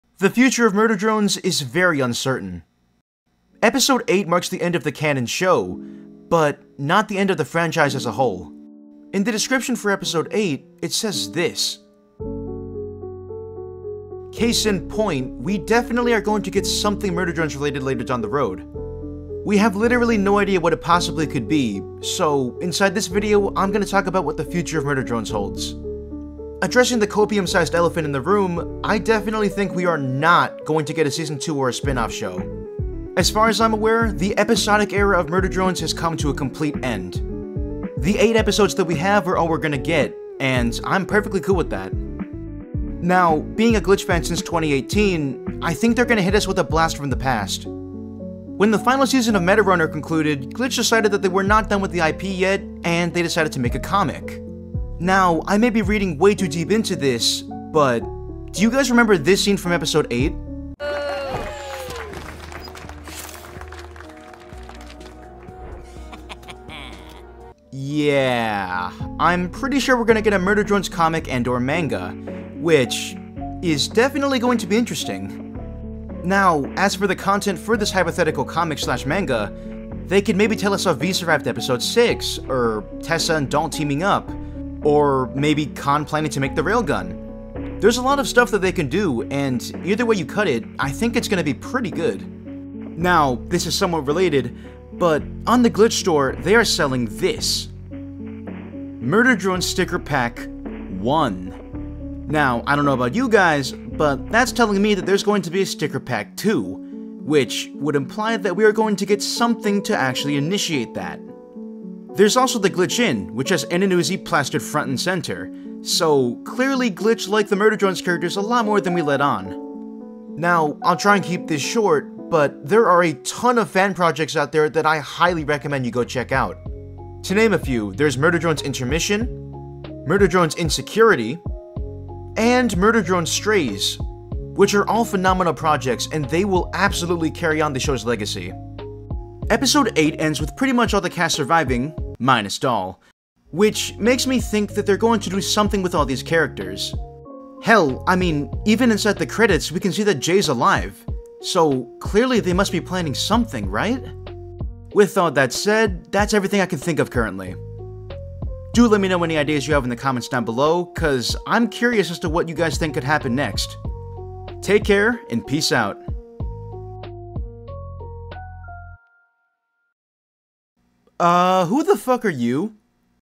The Future of Murder Drones is VERY Uncertain
Episode 8 may be the end of the show, but it isn't the end of the franchise.